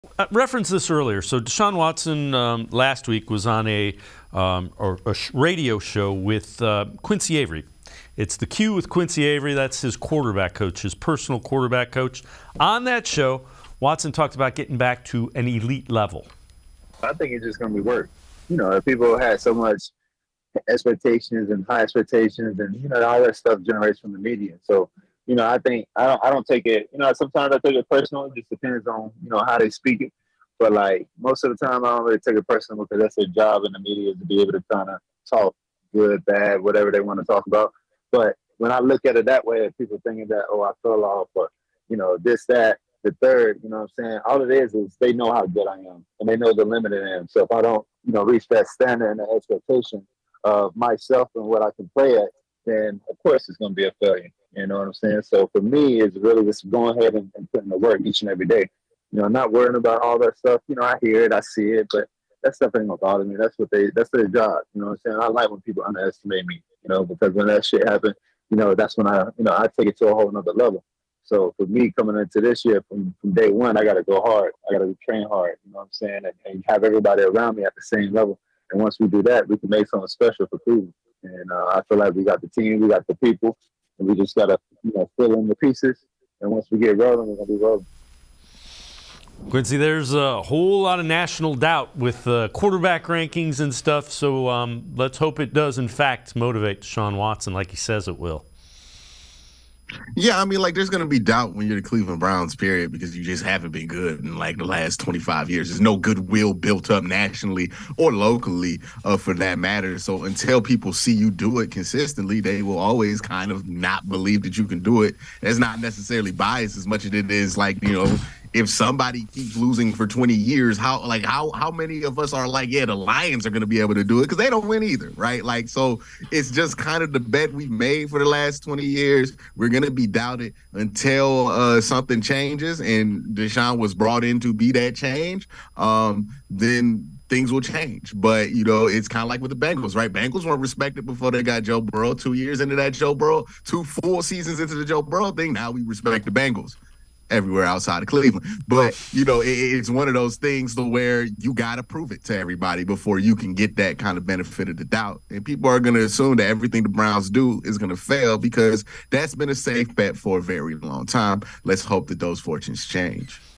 Browns QB Deshaun Watson on Getting Back to an Elite Level - Sports4CLE, 3/2/23
Dave Bacon and Quincy Carrier respond to what Deshaun Watson had to say on getting back to an elite level.